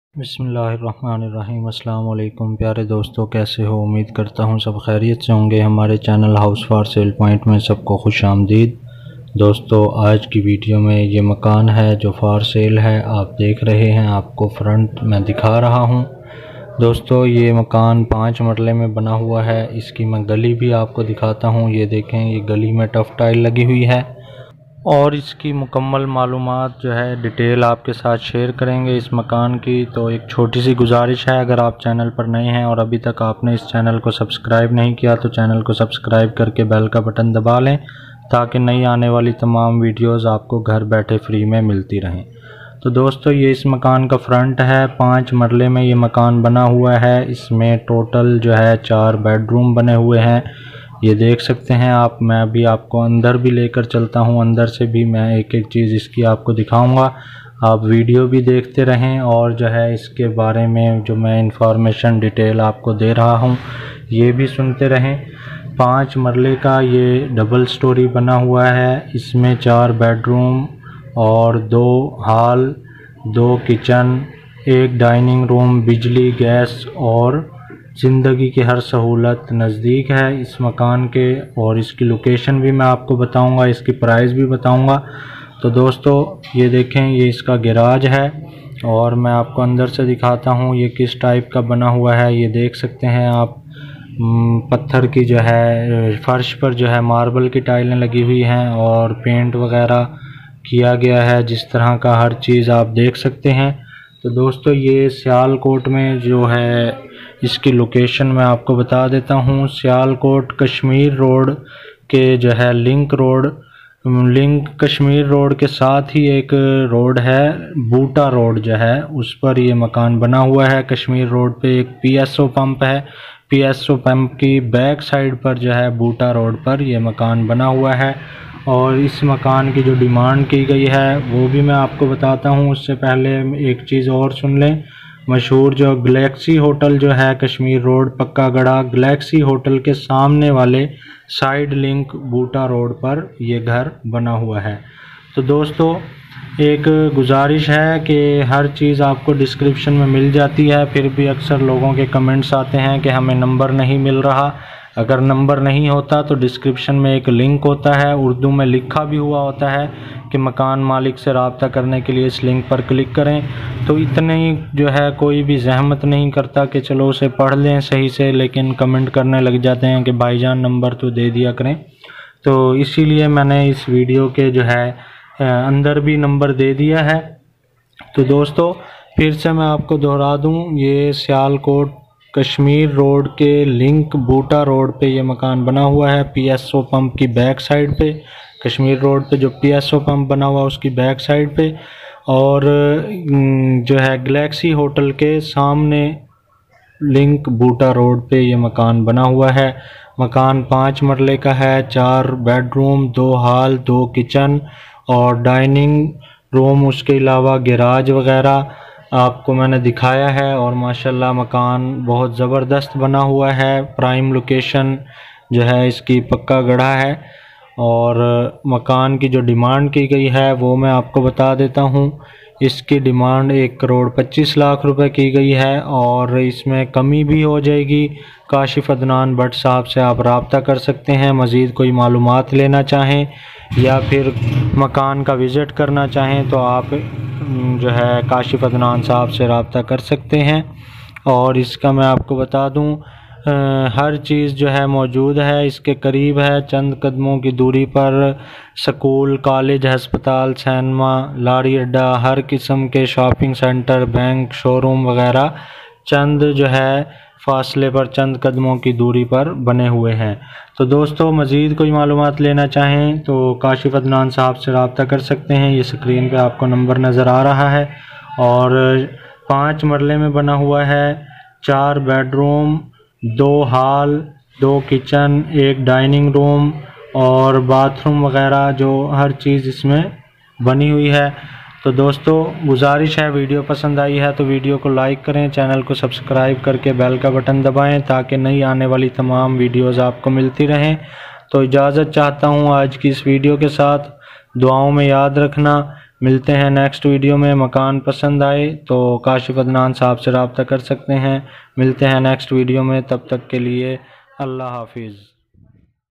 House For Sale 5 Marla Near Kashmeer Road Sialkot || Pso Pump Back Side // Five Marla Double Story
ایک شاندار 5 مرلہ کا گھر ہے
  4 بیڈرومز 2 ہال 2 کچن 1 ڈائننگ، بجلی گیس اور زندگی کی ہر سہولت نزدیک موجود ڈیمانڈ صرف 1 کروڑ 25 لاکھ ( کمی ہو جائے گی)
یے مکان کشمیر روڈ کے لنک بوٹا روڈ پے بنا ہوا ہے
کشمیر روڑ پکا گڑھا گلیکسی ہوٹل کے سامنے والی سائڈ لنک بوٹاروڑ پر یہ گھر ہے،
کشمیر روڈ پر پی ایس او پمپ کی بیک سائیڈ بوٹا روڈ پر
کوئی ایسی سہولت نہیں جو وہاں موجود نہ ہو، سکول، کالج، ہسپتال، سینیما ، لاری اڈہ ، ہر قسم کے شاپنگ سنٹر،  بینک ، شوروم،
 یہ سب چند قدموں کی دوری پر واقع ہے
مزید معلومات کے لیے رابطہ کریں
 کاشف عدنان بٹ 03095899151